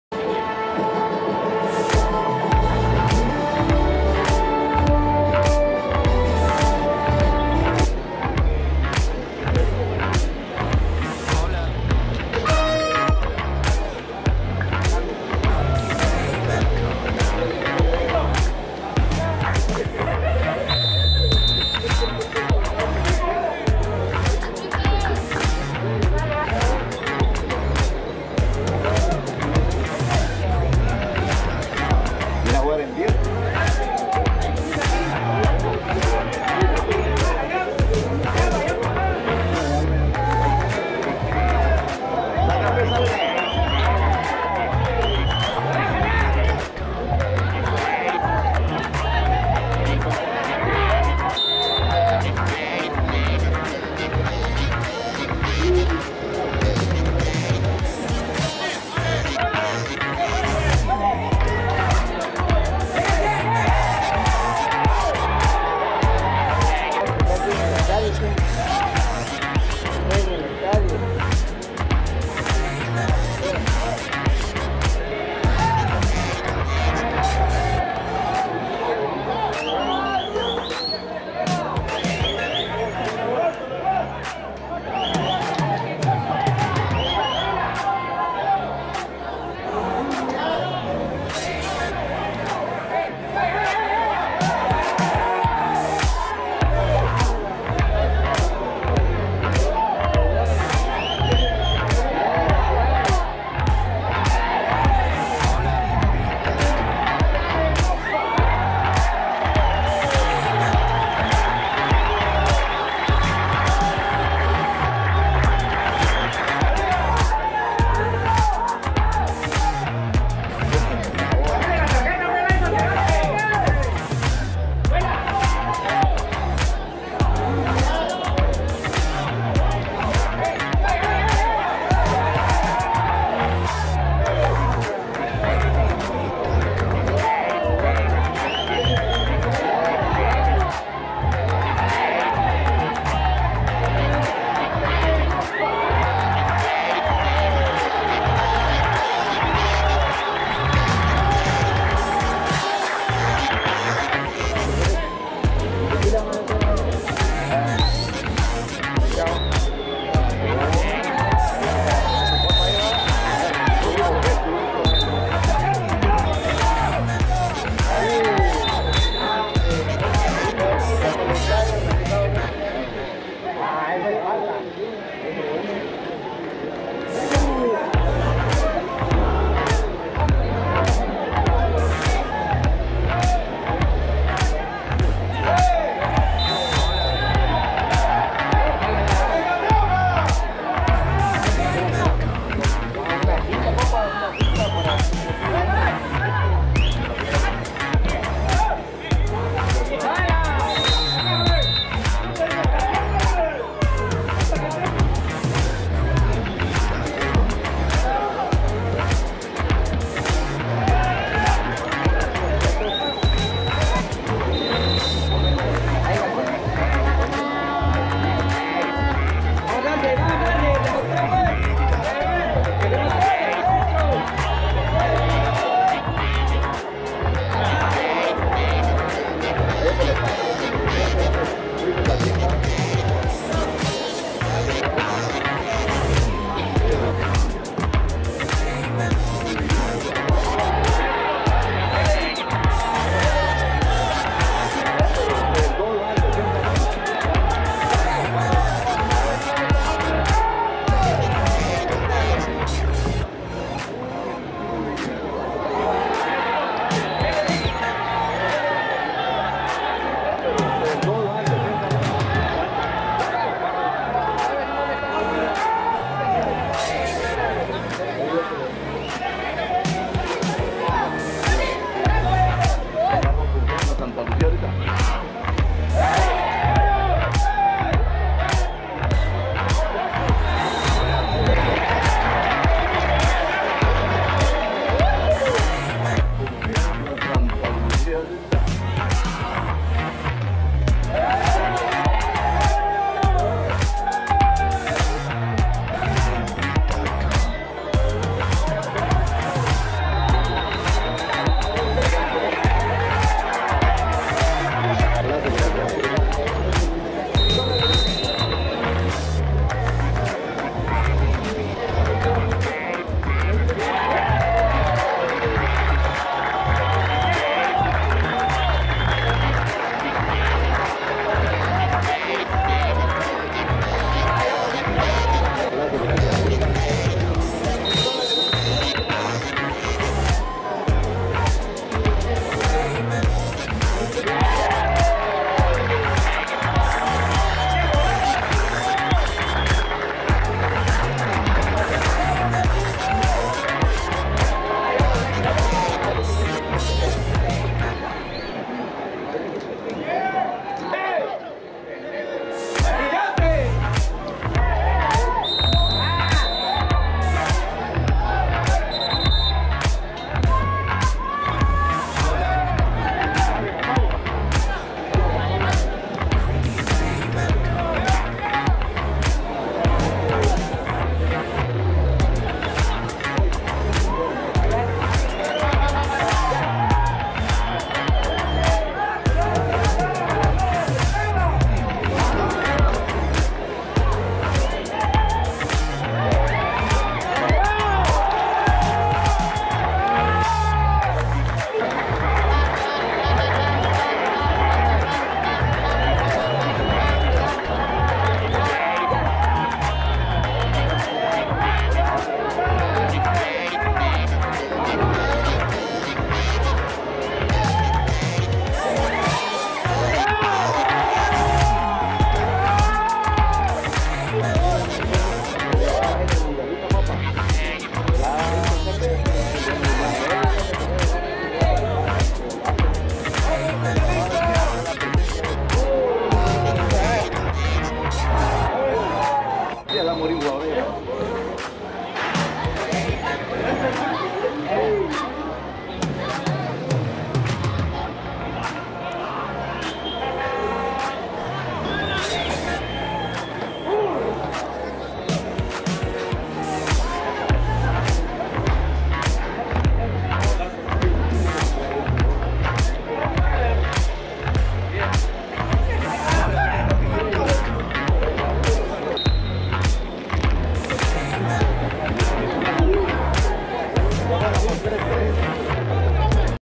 Deportivo Malacateco 3 - FC Santa Lucía 1 Jornada 4 Apertura 2022
Deportivo Malacateco 3 - FC Santa Lucía Cotzumalguapa 1
Jornada 4     Apertura 2022
Estadio Santa Lucia, Malacatán